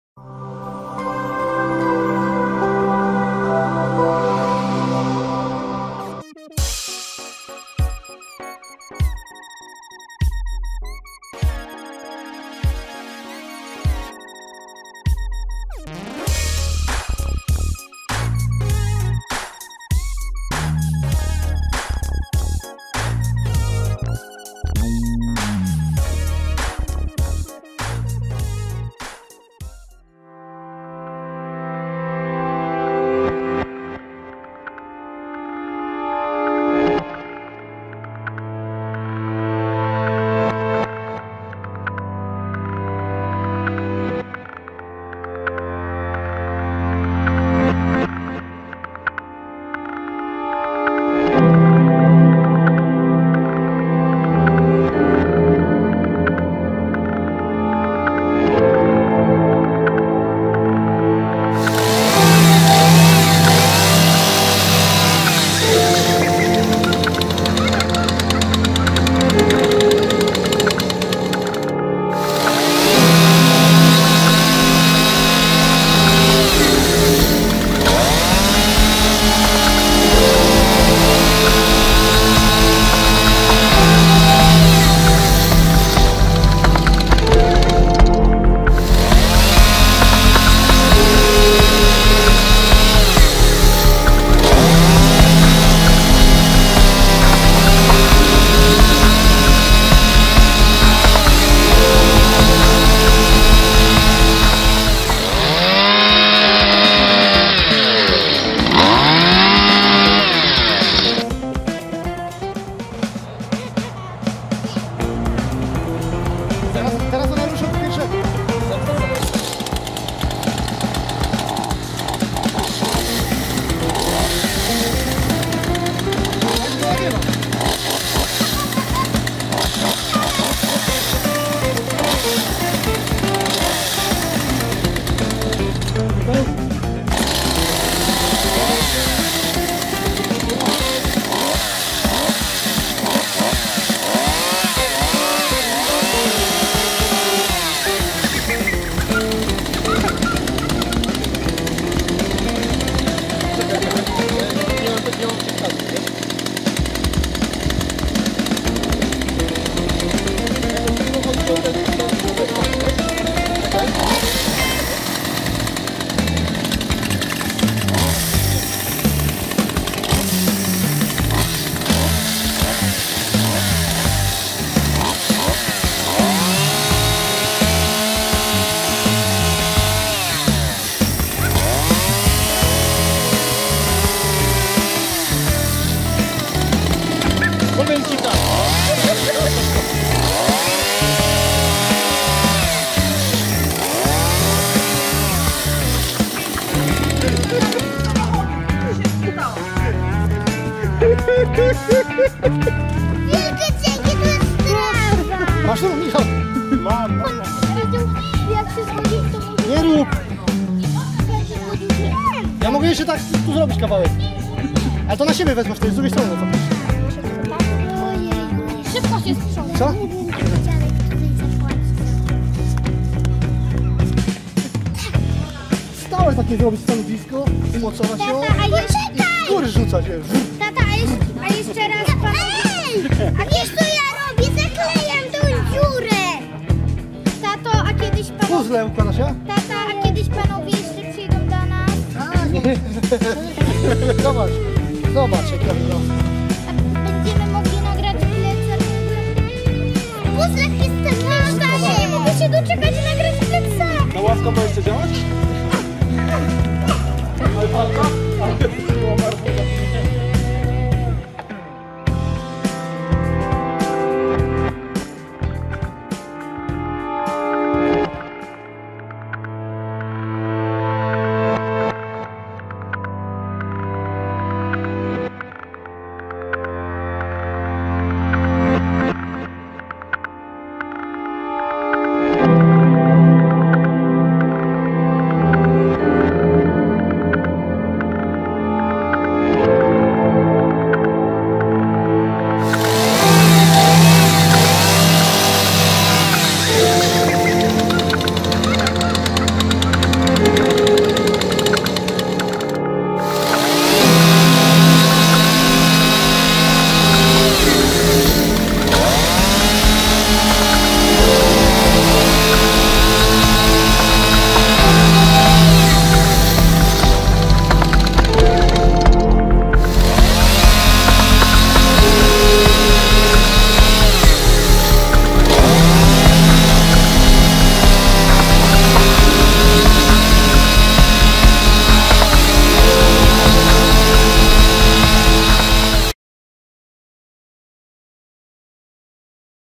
Anka ma puzzle z arbuza ;) - KROIMY GO PIŁĄ
mamy arbuza i zrobiliśmy z niego puzzle 3D :) piłą